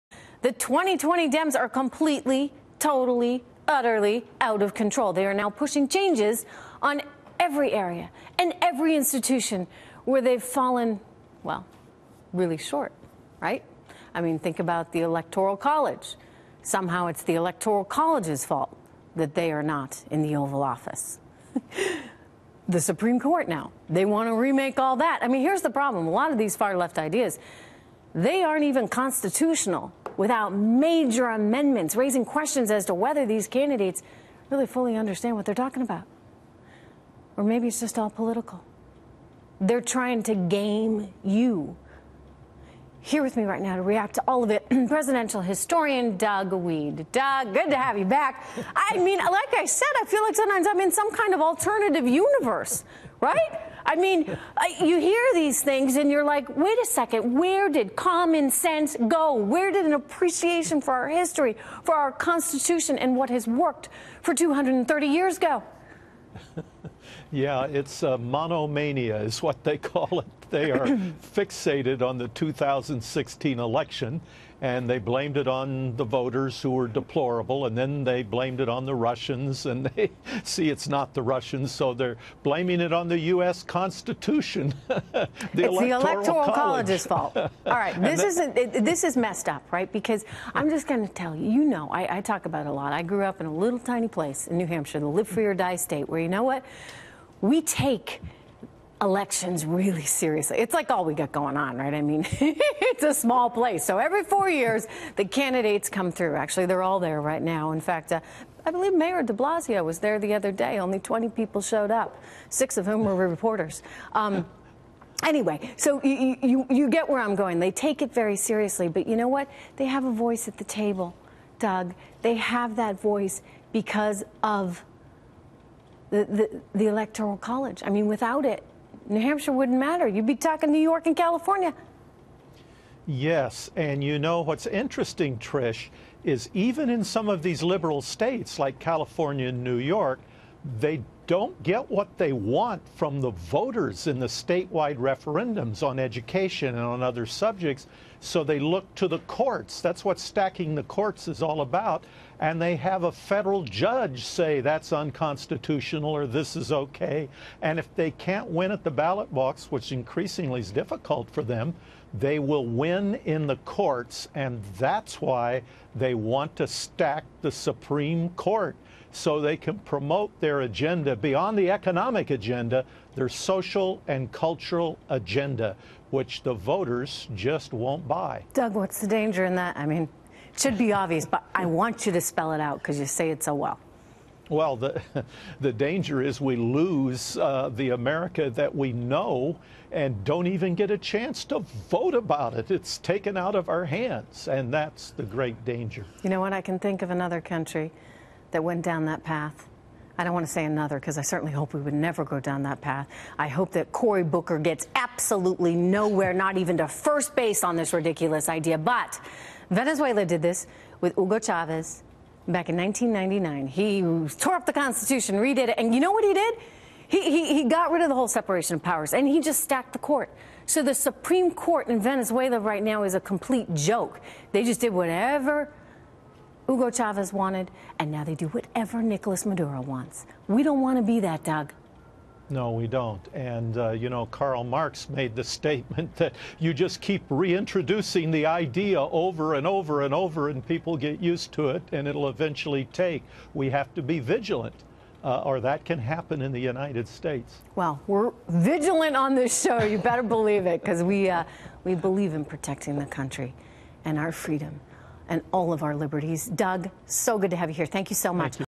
The Strange Democrat 2020 Candidate Platform
In addition to promoting infanticide; eliminating borders and Immigration Customs Enforcement (ICE) and banning guns; the Democrats have all signed on to The Green New Deal, which advocates for: raising taxes, paying people not to work, eliminating air travel, banning cows and rebuilding every structure in America to run on ‘sustainable energy’.

The platform is now expanding to: lowering the voting age to 16; eliminating the electoral college; placing term limits on federal judges; and adding more Justices to the Supreme Court to advance the aforementioned progressive values.